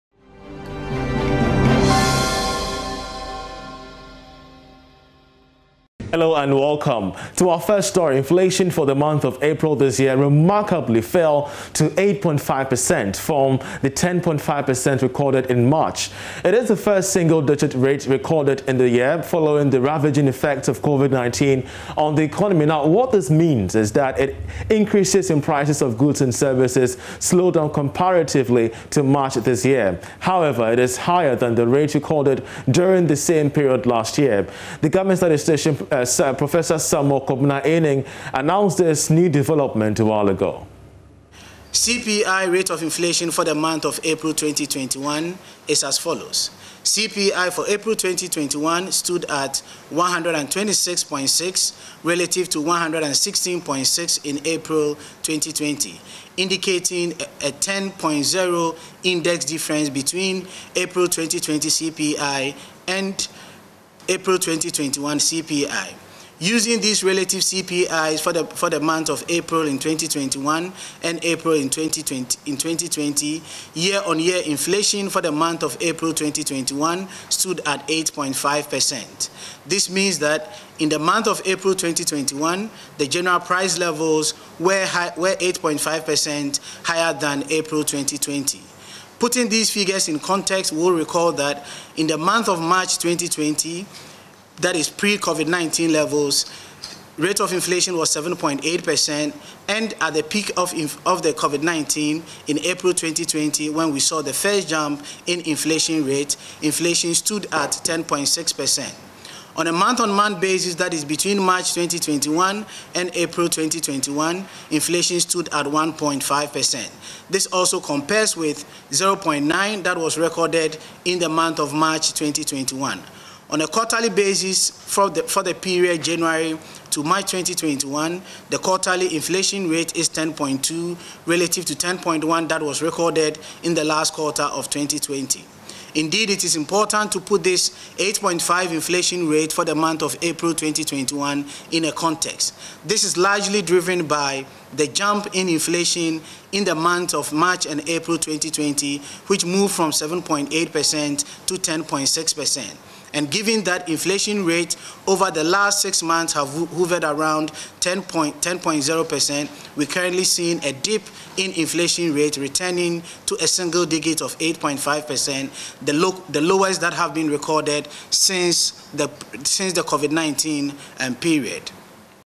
April Inflation: Single Digit Recorded for First in 2021; drops from 10 3% to 8 5% - (12-5-21)
April Inflation: Single Digit Recorded for First in 2021; drops from 10 3% to 8 5% - The Market Place (12-5-21)